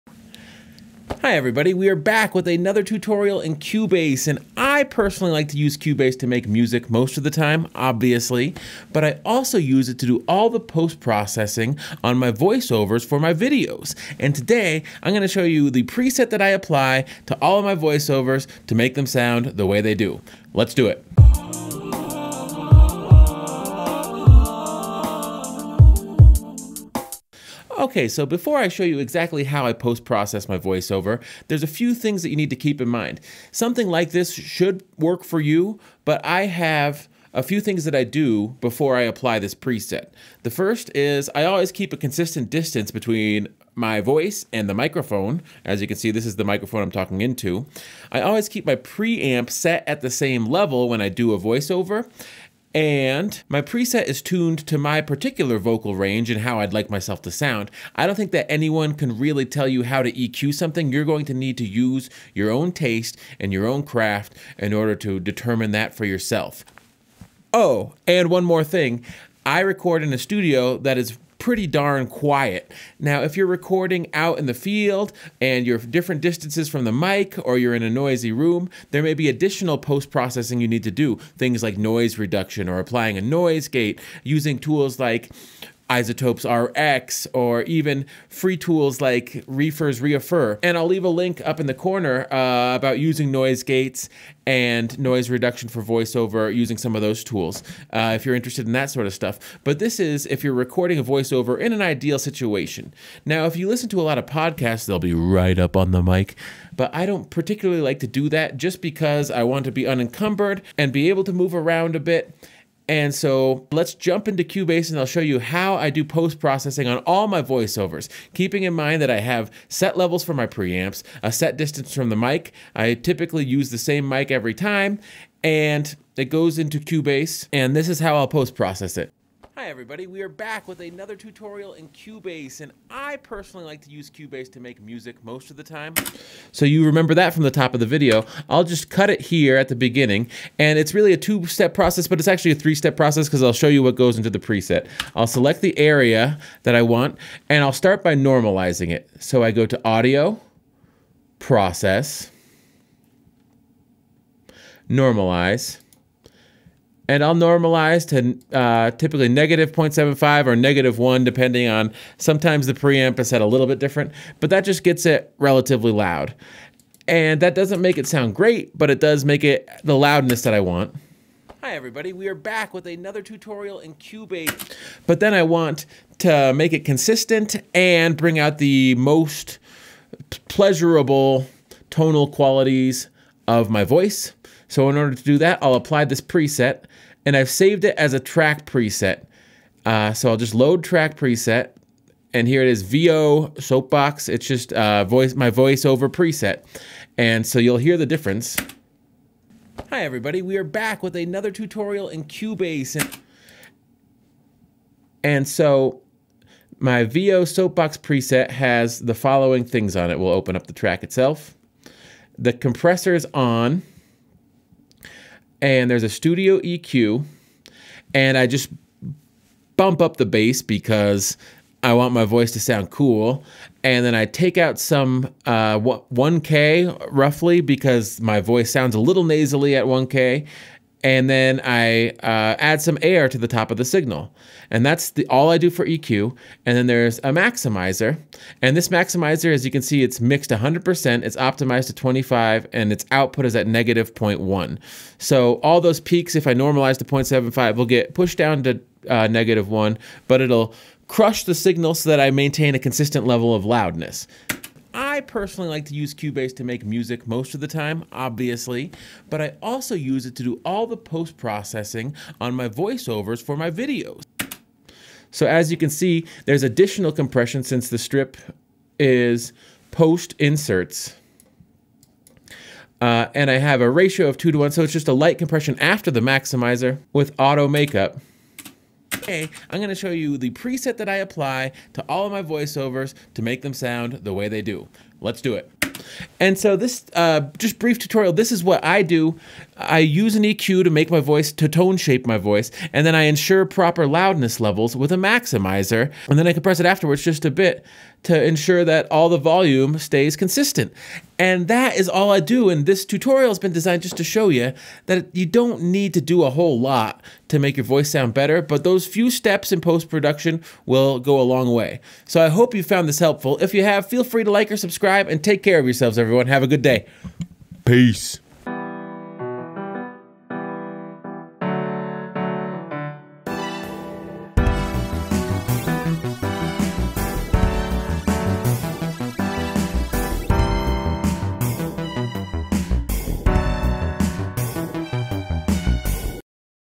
Cubase 9.5 Tutorial: Voice Over Post Processing Using Stock Plugins
Hi everyone!  In this Cubase tutorial, you will learn how to post process voice overs.  This is the method I use and it may not totally apply to what you're trying to do, but using some of these tips and tricks should get you part of the way there.  I hope you find it useful!